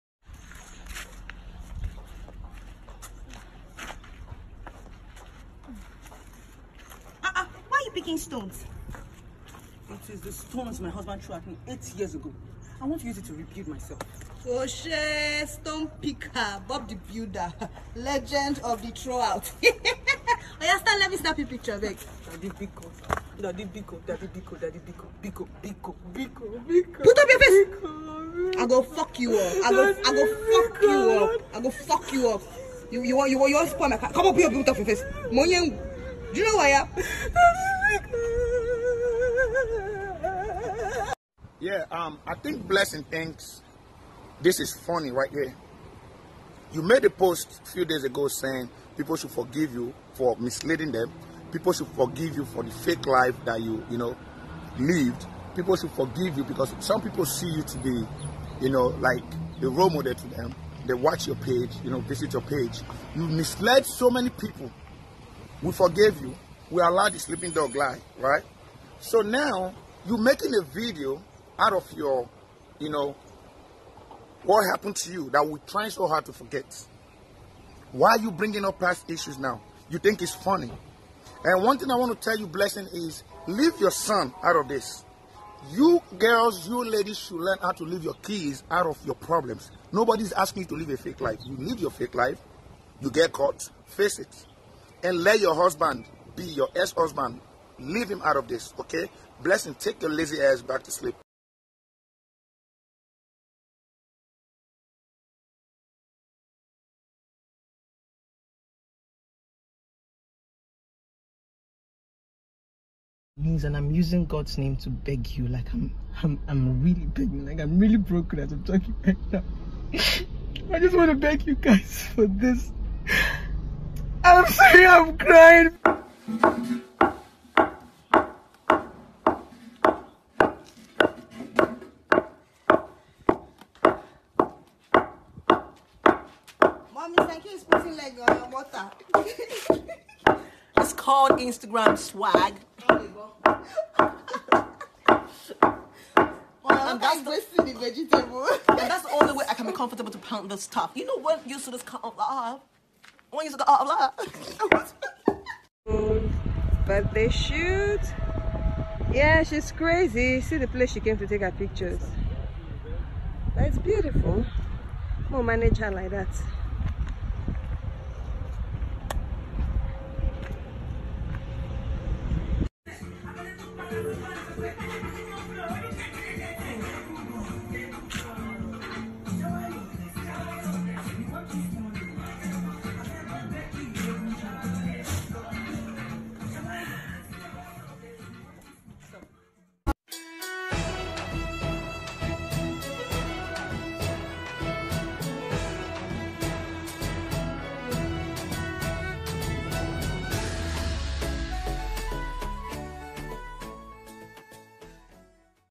INSTAGRAM DRAMA:BlessingCEO Mocks HERself in a bead to prove the story of she and Onyeze was a lie.
Blessing CEO Mocks HERself in a bead to prove the story of she and Onyeze was a lie.

Btw, I thought she said everything was going to die down naturally, why this NOW. .